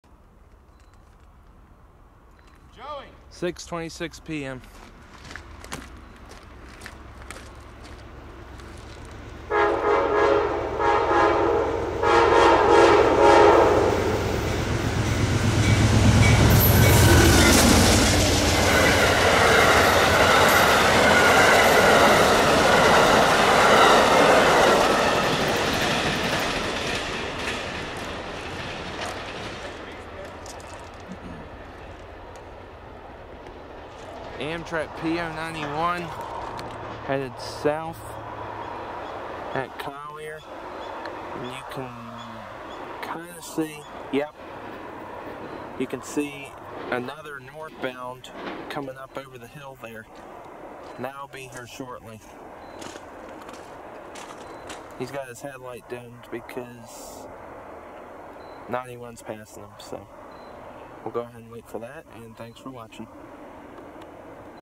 Amtrak P091
6:26 PM

Amtrak's Silver Star flies south. Shot in 4K with a Sony Handycam FDR-AX53.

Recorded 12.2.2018